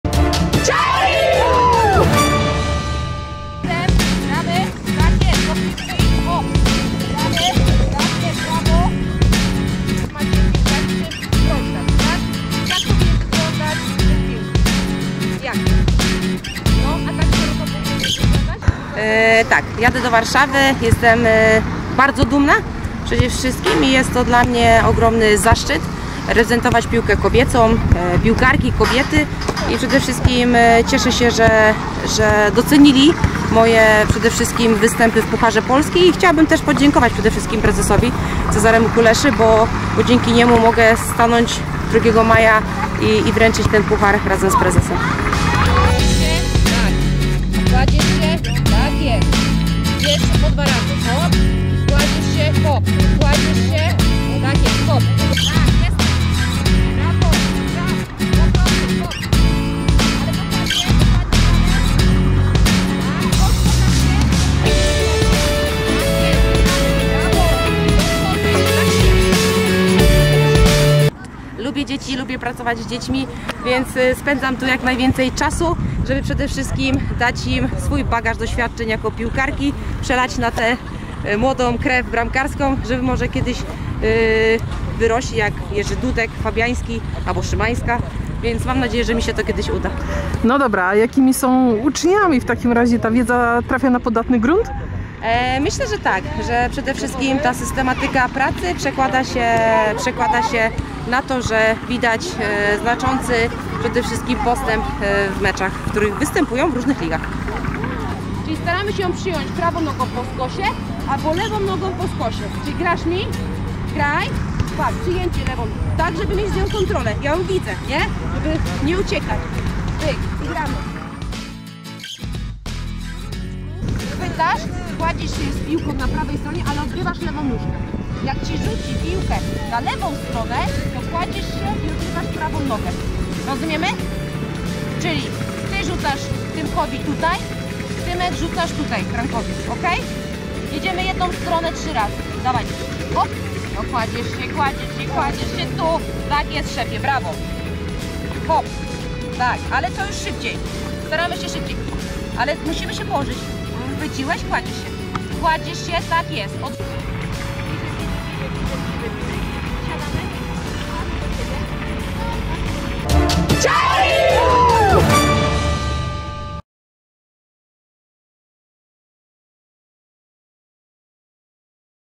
Anna Szymańska w roli trenerki
Drużyna, która 2 maja wygra finał Fortuna Puchar Polski na murawie PGE Narodowego w Warszawie otrzyma trofeum z rąk Ani Szymańskiej i prezesa PZPN Cezarego Kuleszy. Po raz pierwszy w historii tych rozgrywek kobieta wręczy puchar męskiemu zespołowi! BRAWO! W naszym nowym filmie wypowiedź Ani oraz jej nie wszystkim znana rola - trenerki młodzieży :) Zobaczcie koniecznie.